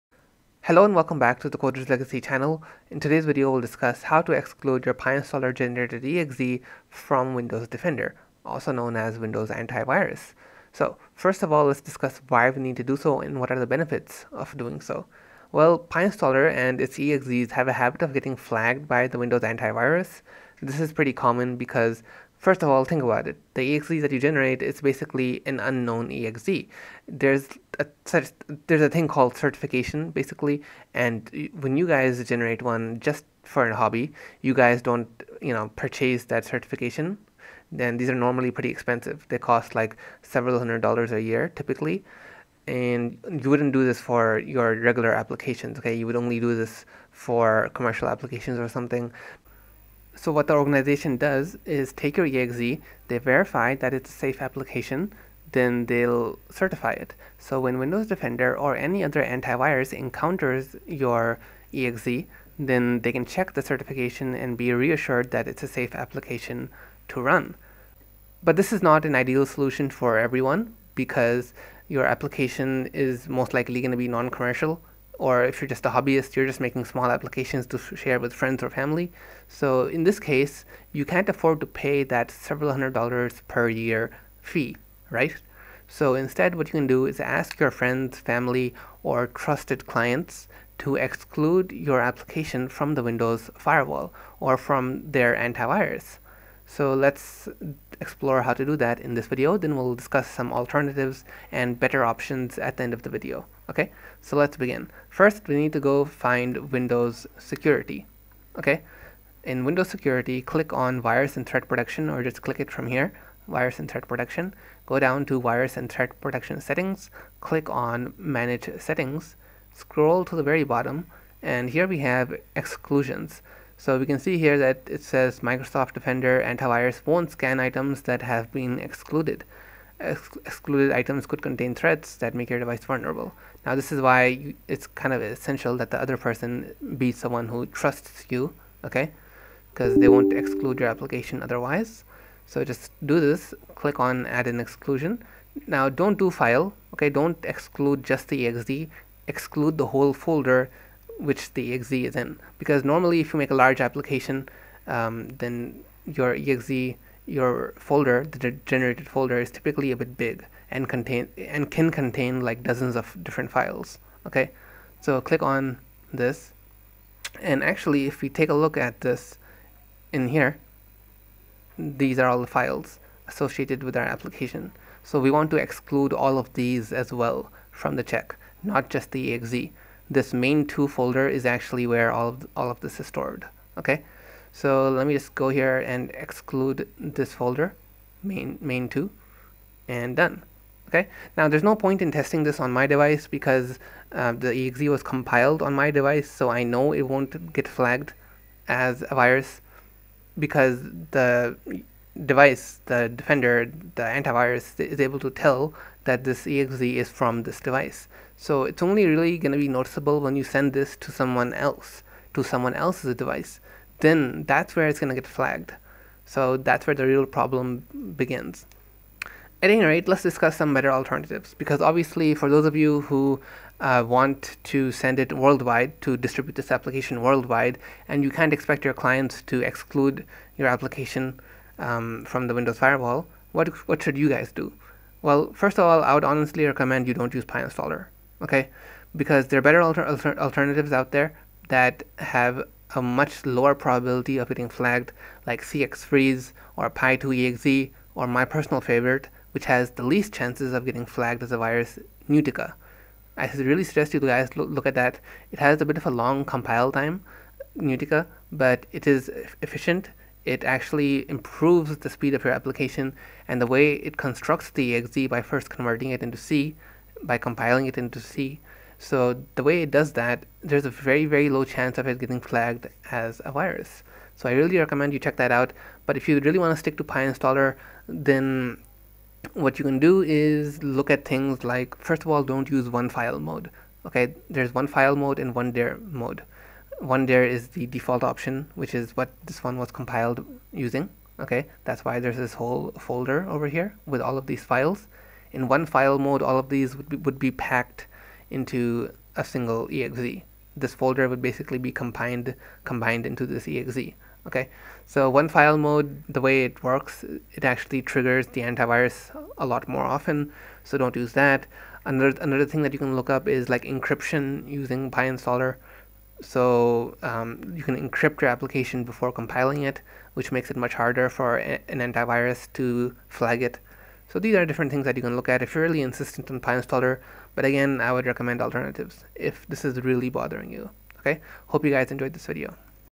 How to Exclude PyInstaller EXE from Windows Defender (AntiVirus)
In this Tutorial we will discuss how to exclude your Pyinstaller .EXE file from the Windows Defender Anti-virus scans and checks. Most importantly, we will discuss the benefits of doing so, and also suggest some alternatives to Pyinstaller.